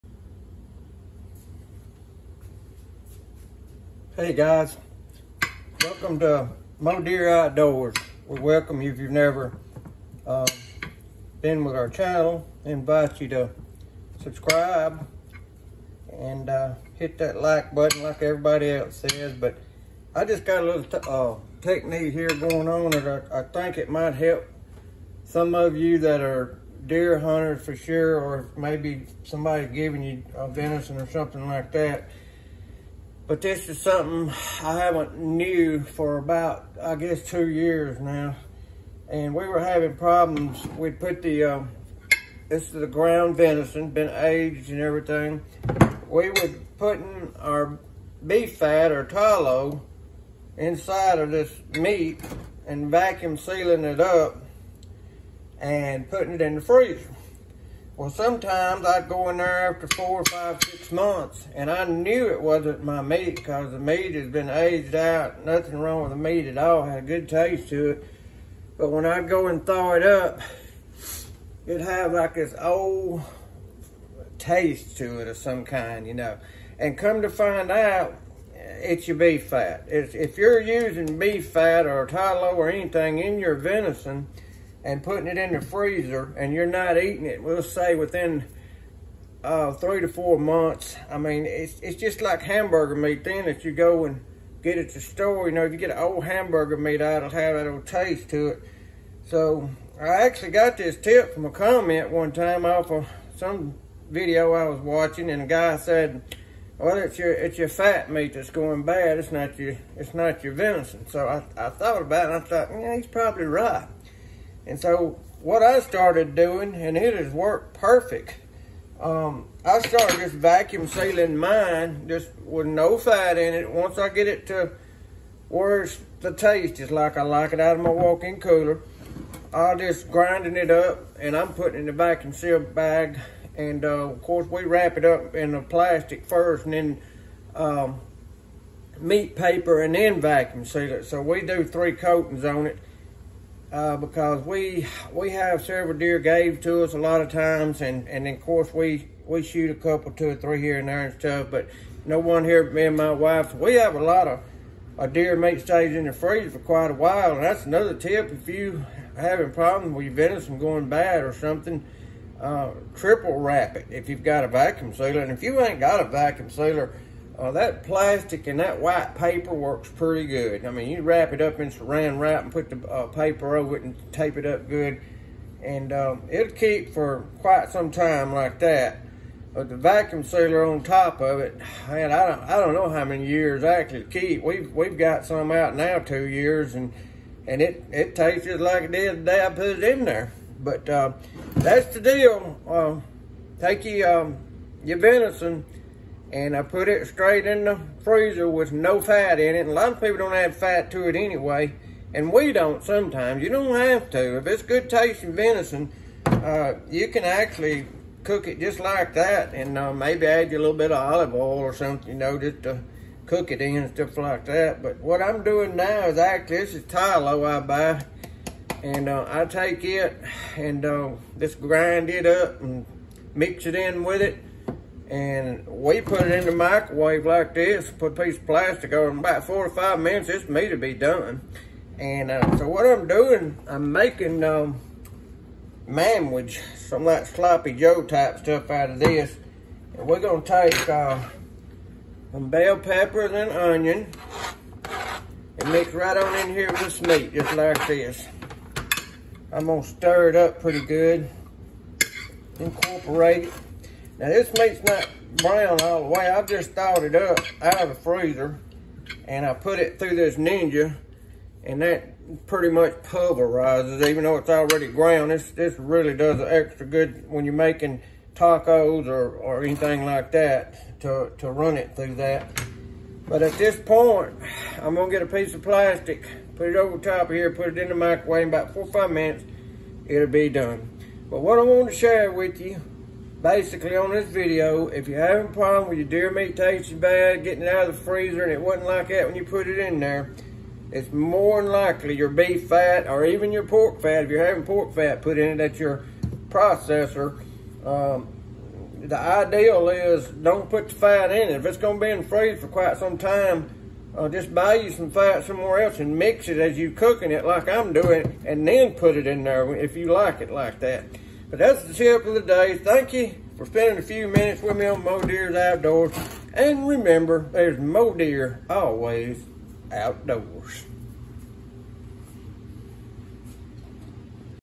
This is possibly why your venison is tasting bad after being in the freezer for awhile.
If you are having problems with your venison tasting bad after being frozen, this might be your answer?